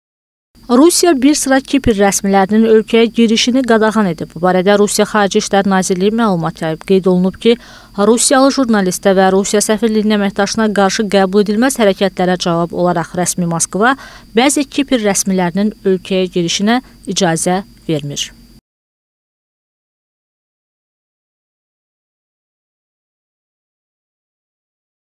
Rusiya bir sıra Kipr rəsmilərinin ölkəyə girişini qadağan etdi - APA TV
Rusiya bir sıra Kipr rəsmilərinin ölkəyə girişini qadağan edib.
Bu barədə Rusiya Xarici İşlər Nazirliyi məlumat yayıb. Qeyd olunub ki, rusiyalı jurnalistə və Rusiya səfirliyinin əməkdaşına qarşı qəbuledilməz hərəkətlərə cavab olaraq rəsmi Moskva bəzi Kipr rəsmilərinin ölkəyə girişinə icazə vermir